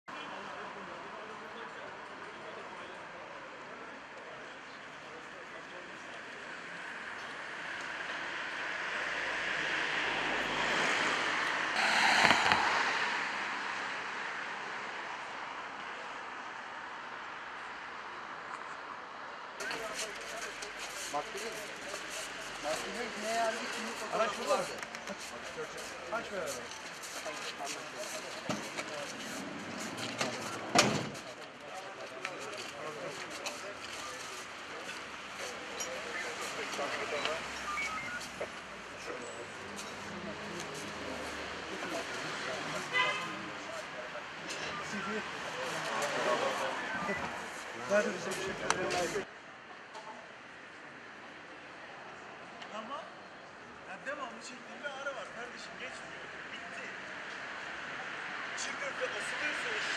Karşıyaka'da esrarengiz cinayet
İzmir'in Karşıyaka ilçesinde 26 yaşındaki Turgay Bahadır, kimliği belirsiz bir arkadaşı tarafından bıçaklanarak öldürüldü.

İzmir,Karşıyaka,Cinayetvideosu,video,haber,haberi,videolu haber